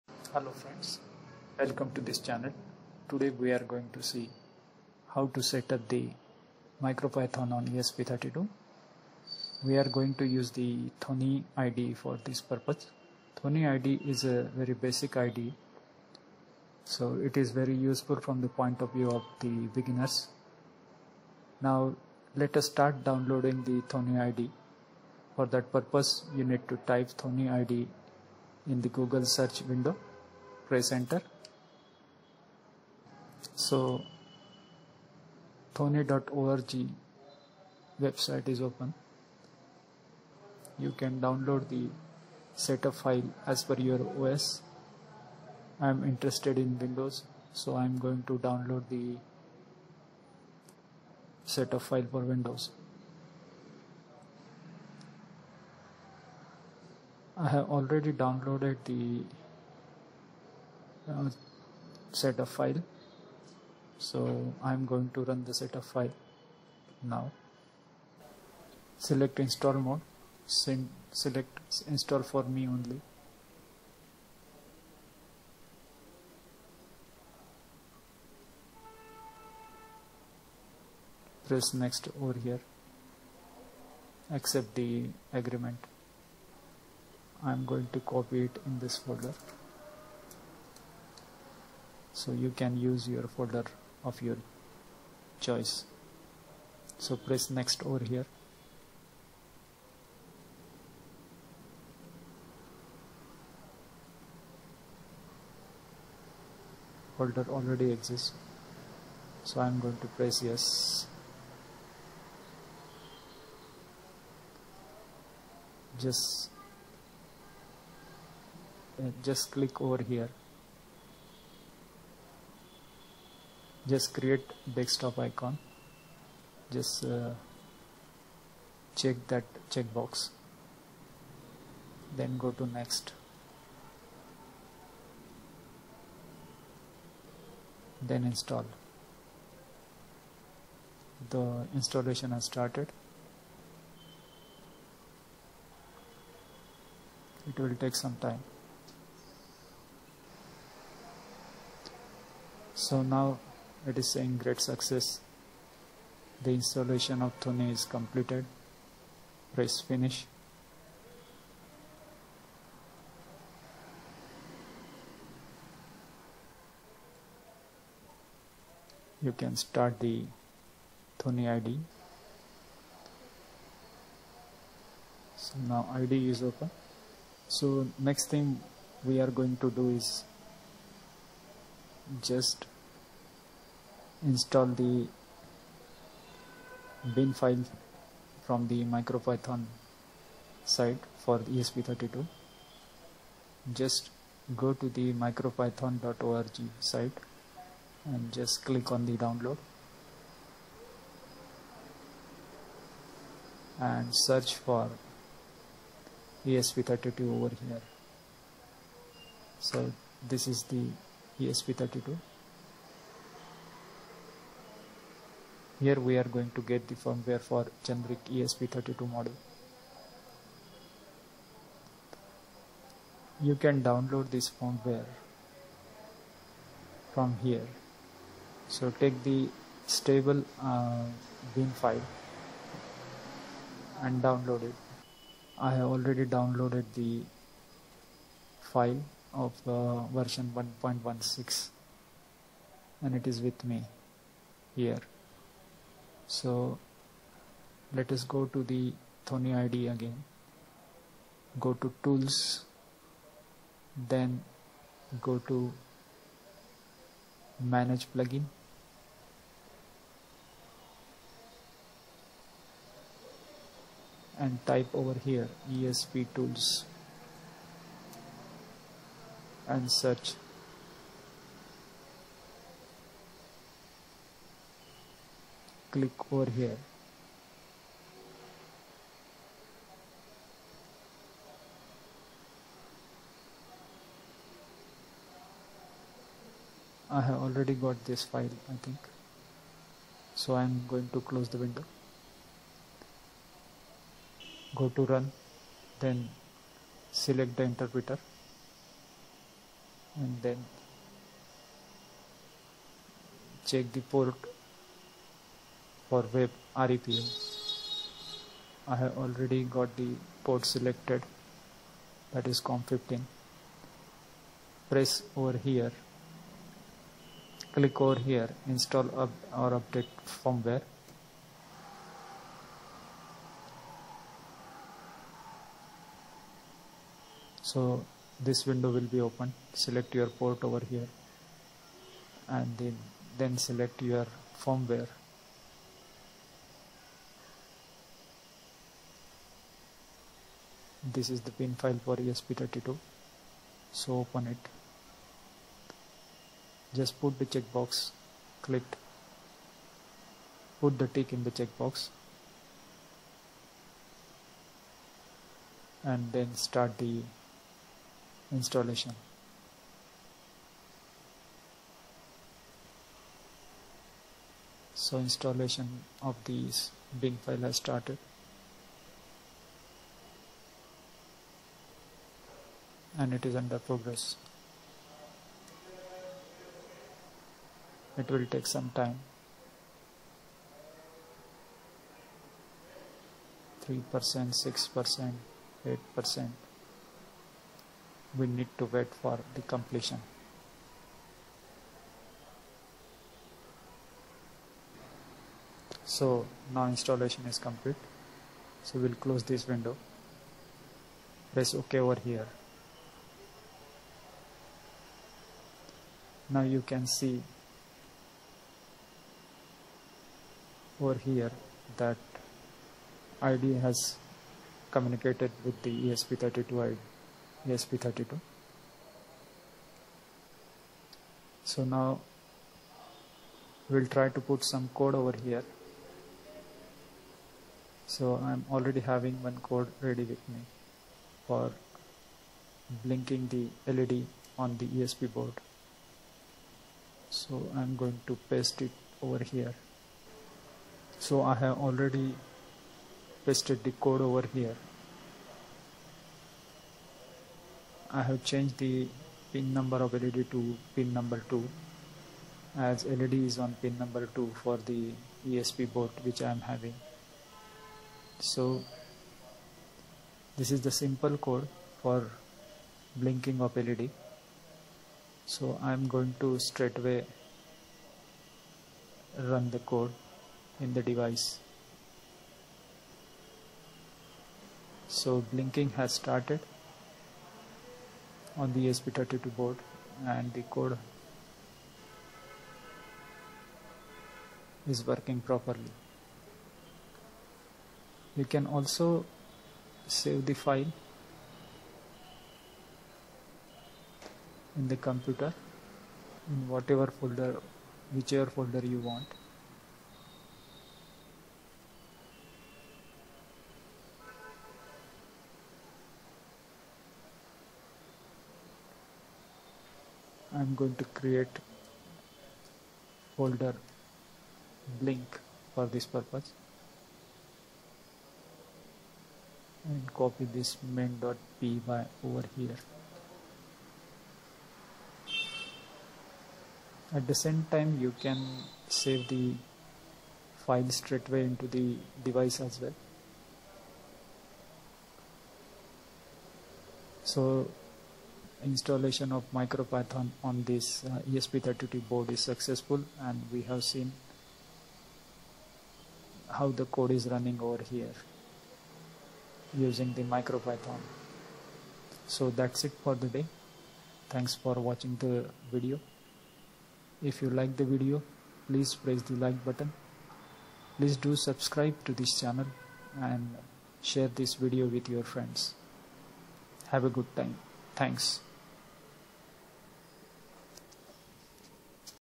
MicroPython Setup on Esp32|Thonny IDE
This videos shows the installation of micropython on ESP32.The Thonny Ide is used for it.